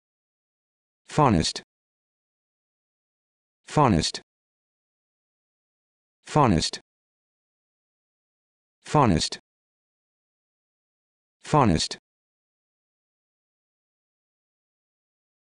How to Pronounce Funnest
This video shows you how to pronounce Funnest